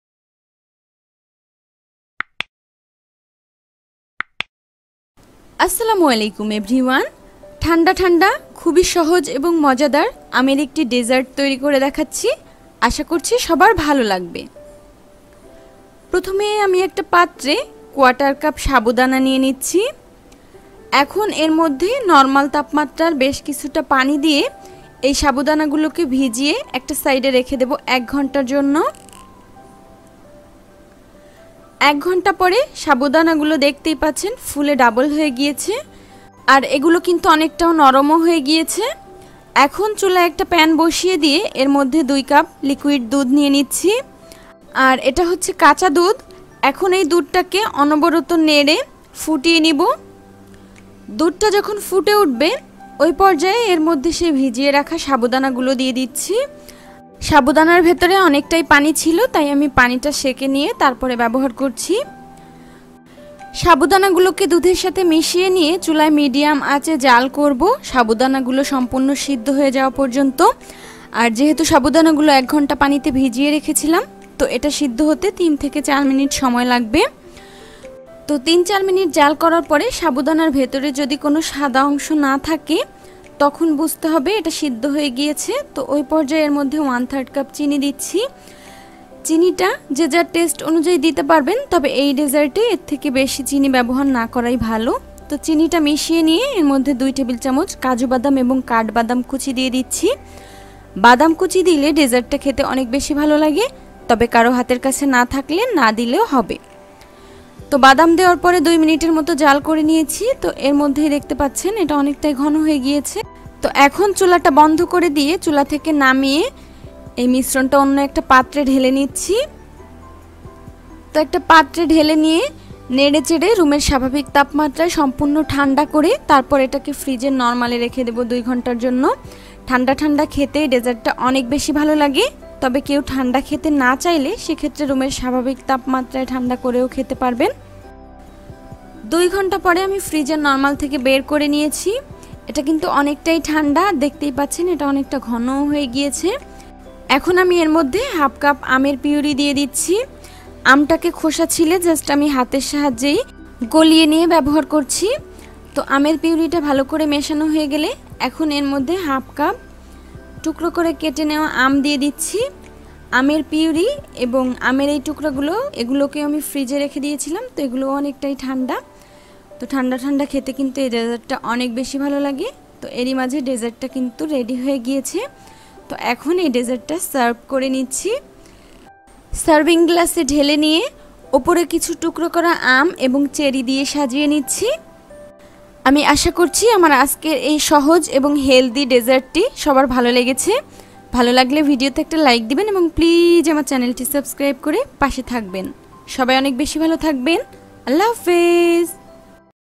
ঠান্ডা ঠান্ডা সাবুদানা এবং আমের ডেজার্ট | Sabudana and Mango Dessert | Easy Dessert | Summer drinks
ঠান্ডা ঠান্ডা সাবুদানা এবং আমের ডেজার্ট | Sabudana and Mango Dessert | Easy Dessert | Summer drinks recipe

Ingredients👇

liquid milk 2 cup
sugar 1/3 cup
Kaju+pesta 2 tbsp
mango puree 1/2 cup
mango cube 1/2 cup